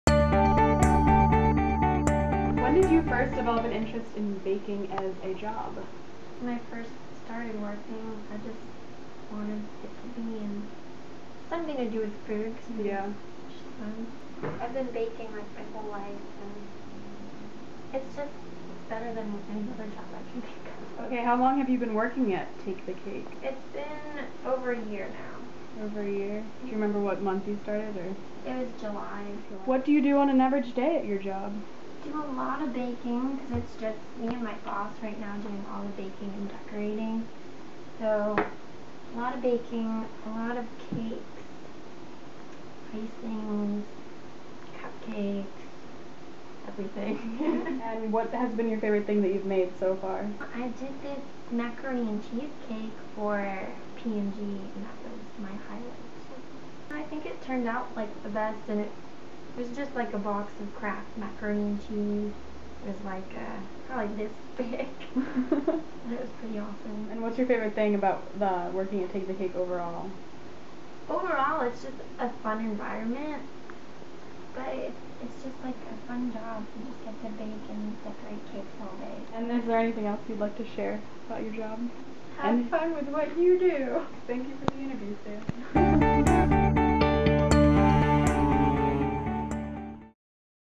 Samantha Zubli talks about her job at Take the Cake
This is the video project of Michelle Robinson, sophomore journalism student at Ohio University Scripps.

In this video Samantha Zubli tells us about her job as a cake decorator at Take the Cake Bakery. The interview took place in her home, while she was decorating a cake for a friend's party. She tells about her daily duties at Take the Cake, gives insight about her job, and talks about what's been her favorite part of working as a cake decorator. Enjoy!